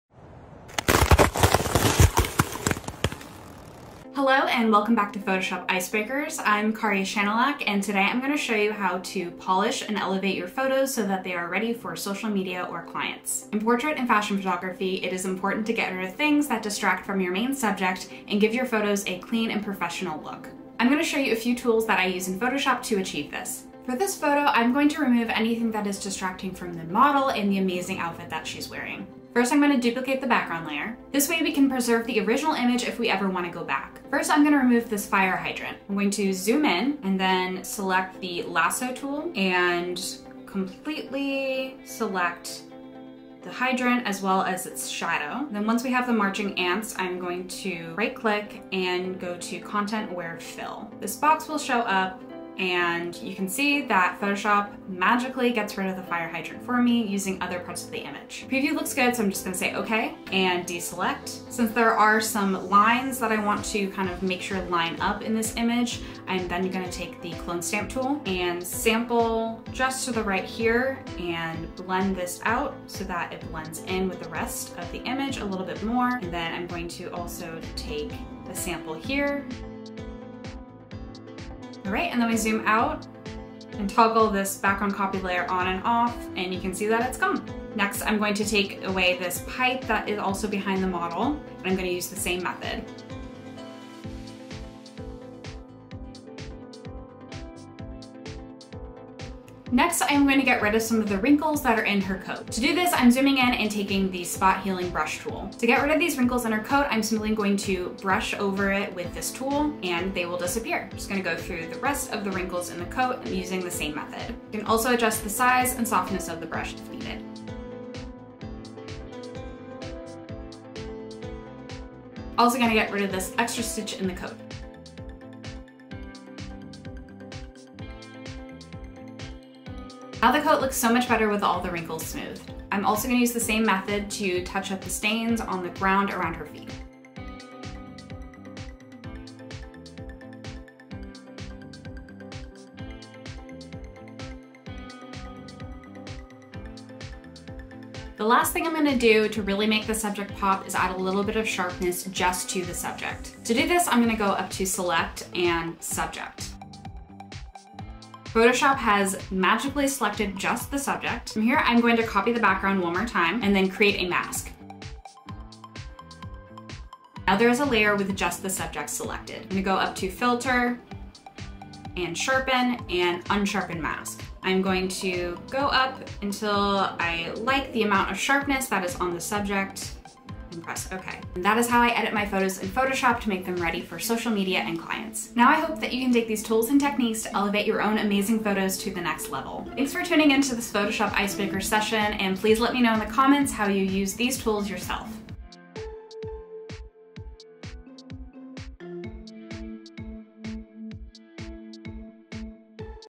How to Edit Photos for Social Media | Photoshop Icebreakers with Karya Schanilec | Adobe Photoshop
In this episode of Photoshop Icebreakers, Karya Schanilec teaches us how to polish and elevate photos in Photoshop to make them ready for social media and clients. She’ll cover topics such as removing objects, retouching and adding selective sharpness to your photography.